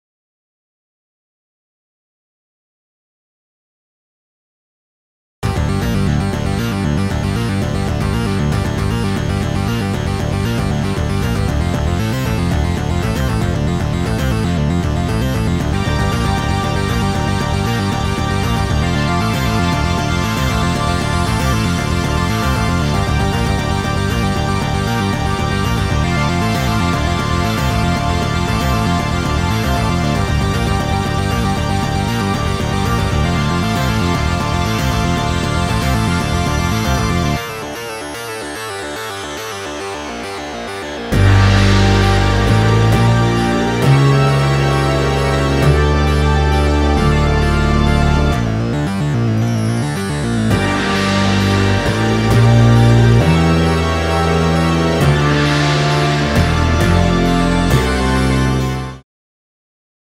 Näin Tehtiin: Itärintama (Jimmyn Viimeinen Rokkenroll Pelastus)
Lyhyt dokumentti kuvituksen työnvaiheista elokuussa julkaistavaan kirjaan "Jimmyn Viimeinen Rokkenroll Pelastus". Tarinan on kirjoittanut ja kuvittanut Jukka Katajamäki.